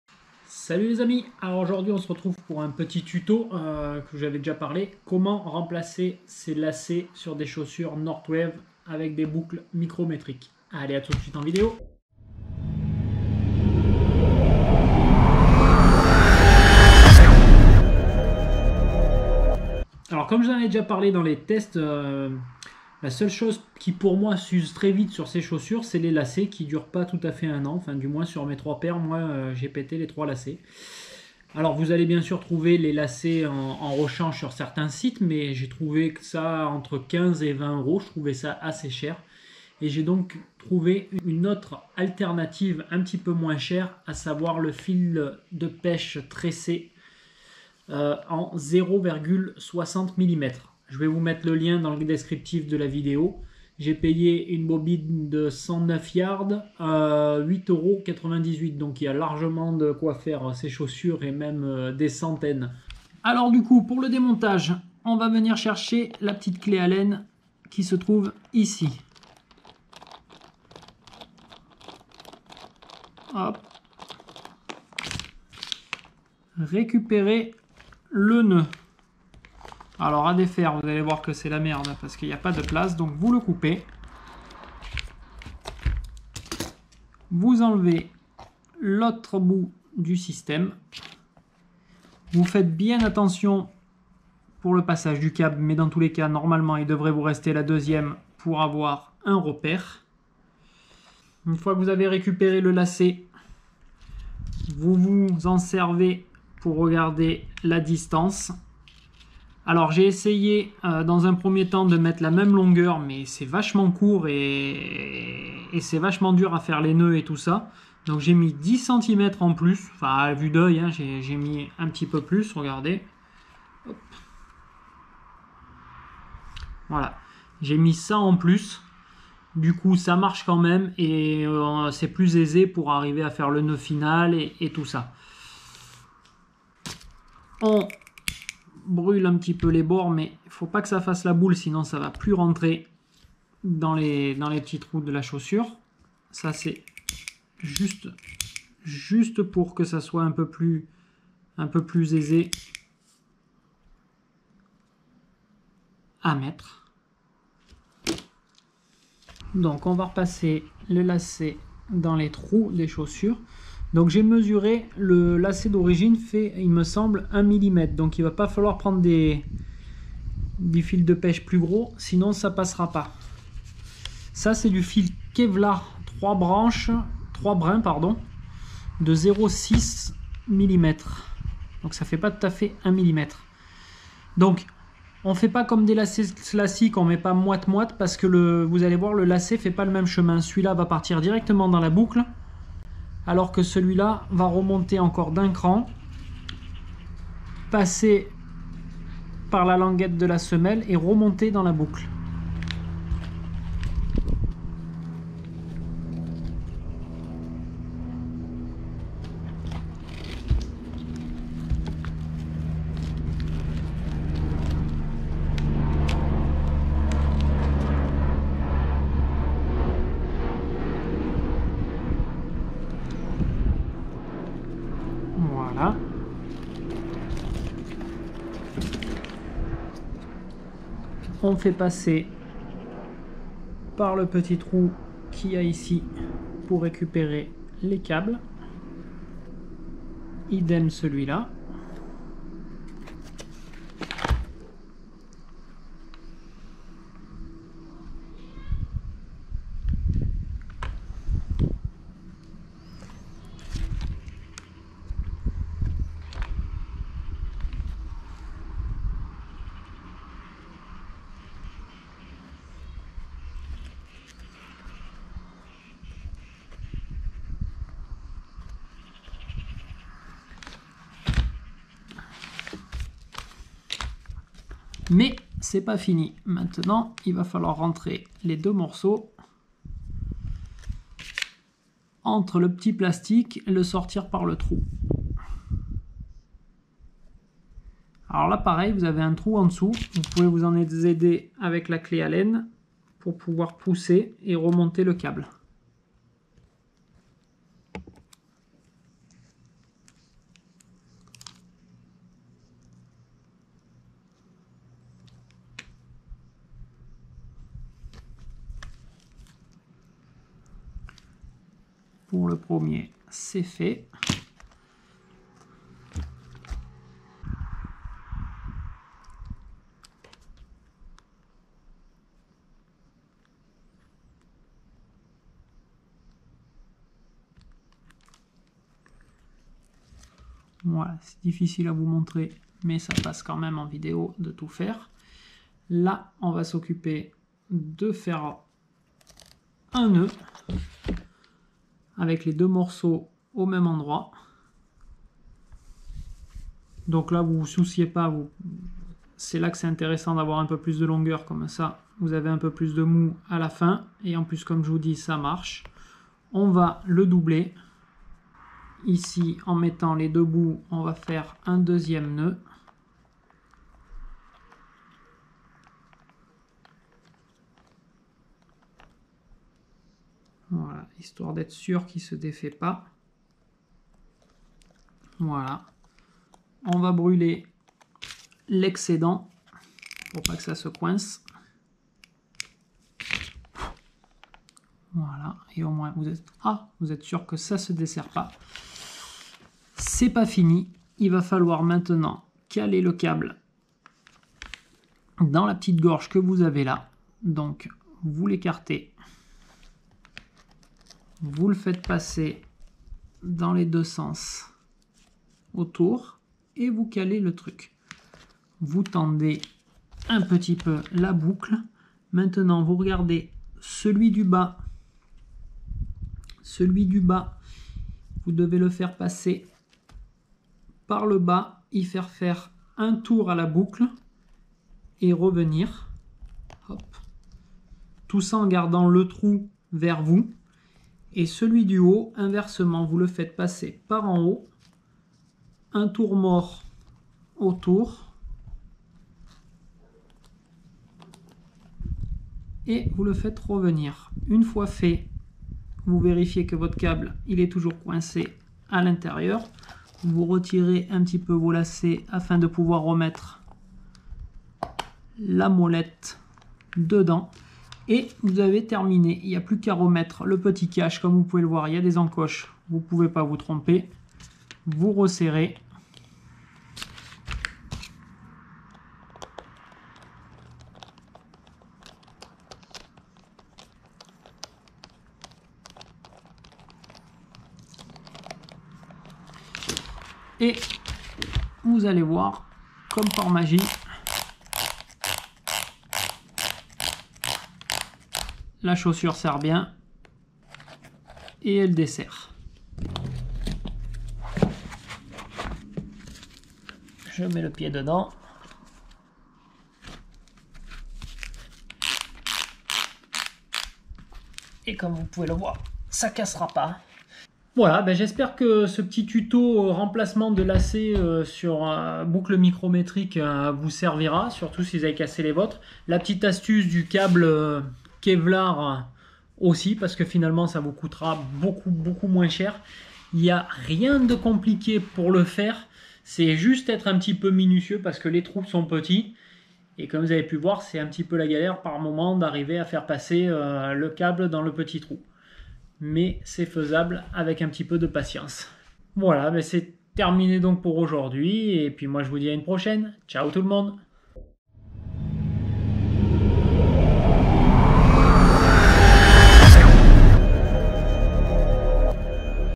TUTO REMPLACEMENT LACET SUR BOUCLE MICROMETRIQUE NORTHWAVE
Salut tous le monde, un petit tuto pour remplacer ses lacets sur boucle micrométrique Northwave a faible coup ,mais aussi toutes les étapes pour le faire.